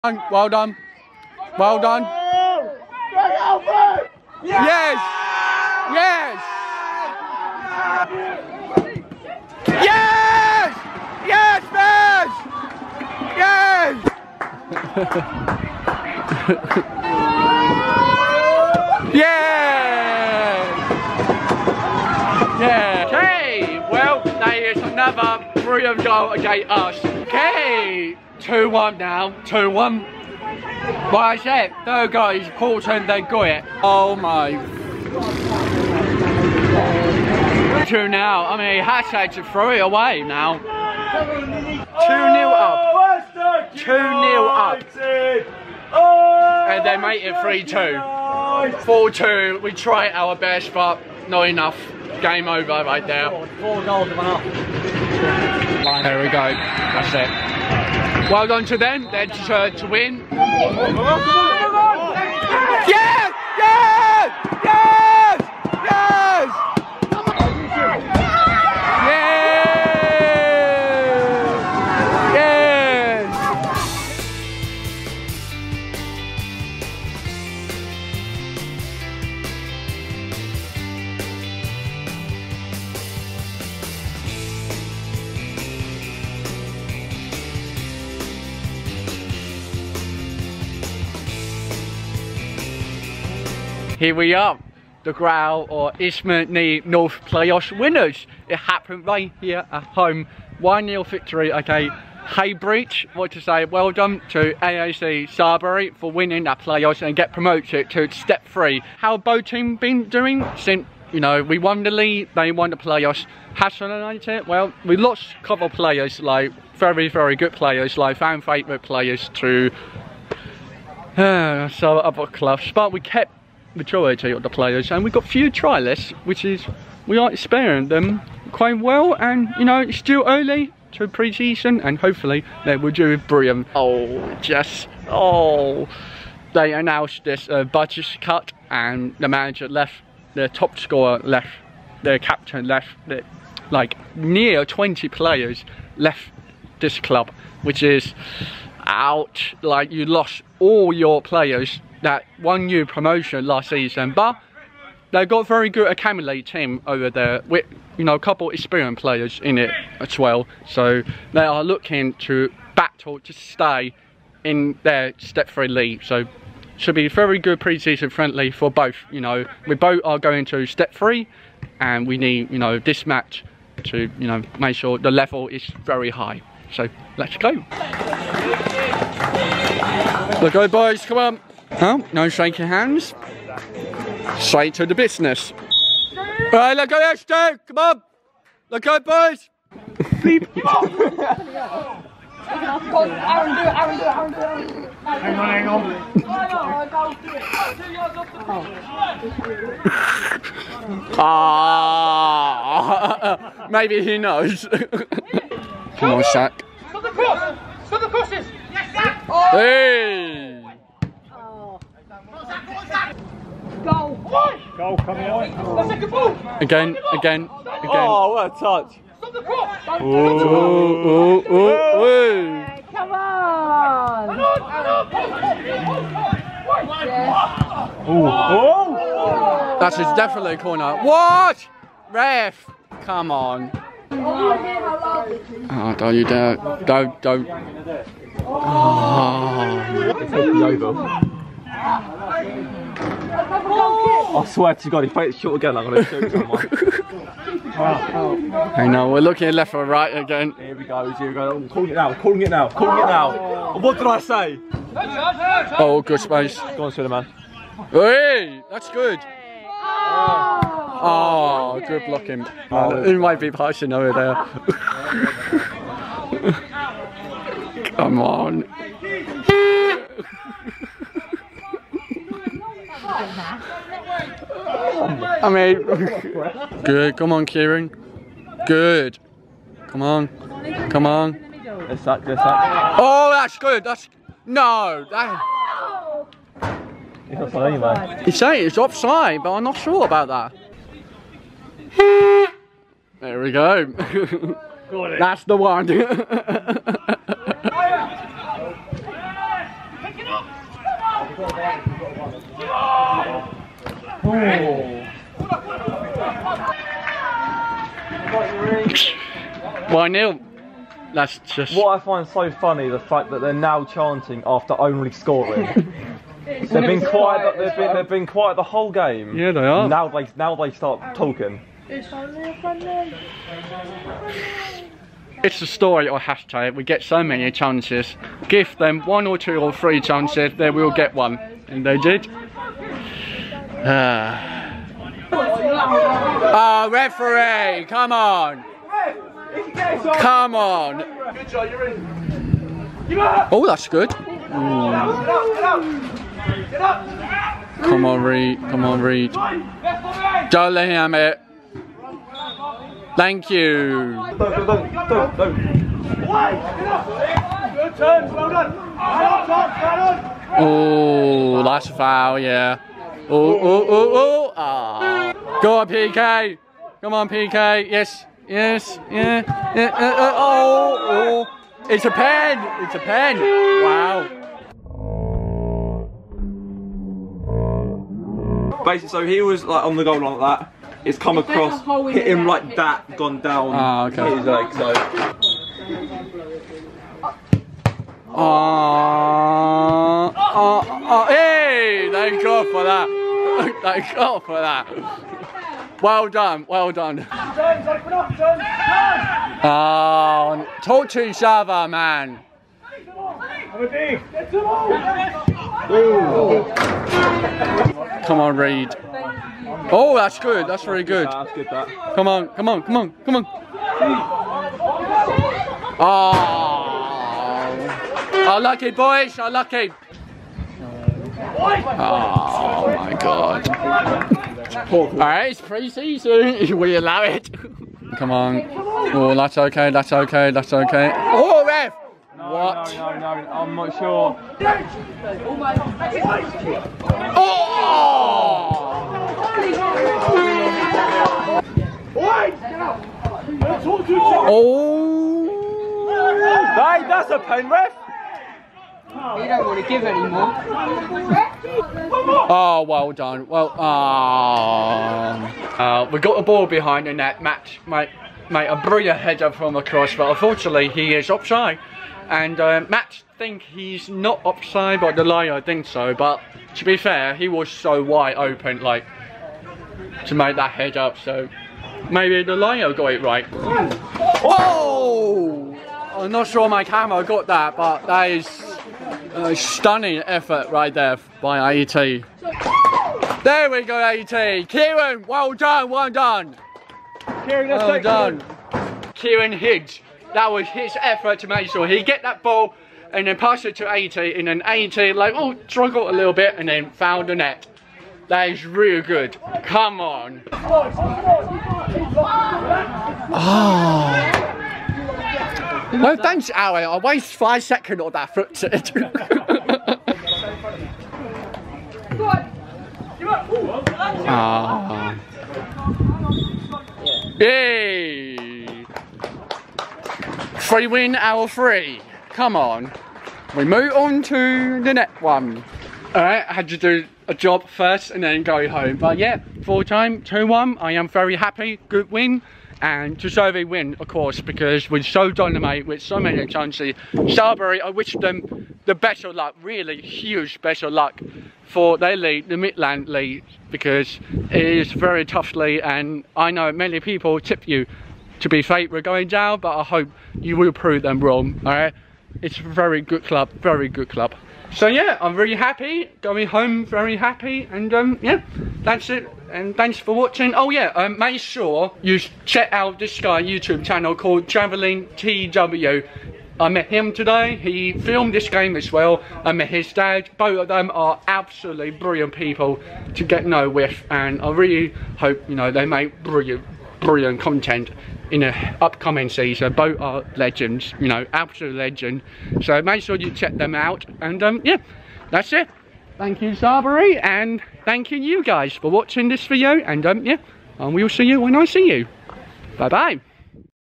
Isthmian League North Champions v Playoffs Winners! - Hashtag United v AFC Sudbury
Matchday Vlog!

I went to Isthmian League North Playoffs Winner AFC Sudbury v my beloved club Isthmian League North Champions Hashtag United! Last season, both teams are even-even during the winter to spring period at top two and eventually, Hashtag came out on top!
This is a preseason friendly game as both sides are playing Step 3 this season and high standards are required!